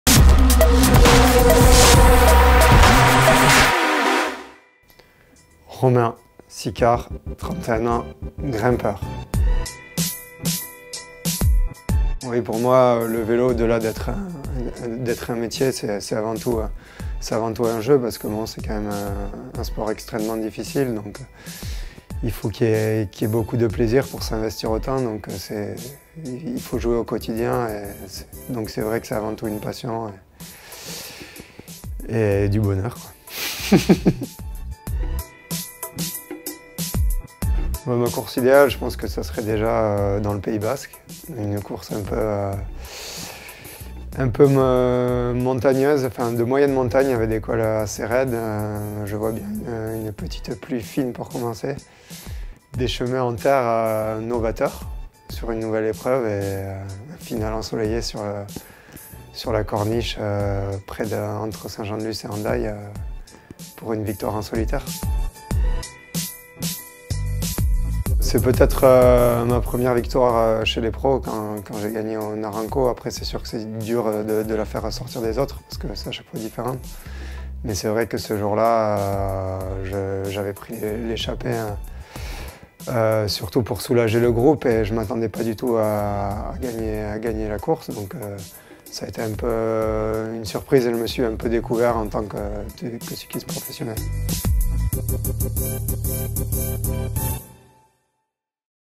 [Portraits 2020] Romain Sicard
Son amour pour le vélo, sa course idéale, sa plus belle victoire, découvrez "le basque" en 3 questions.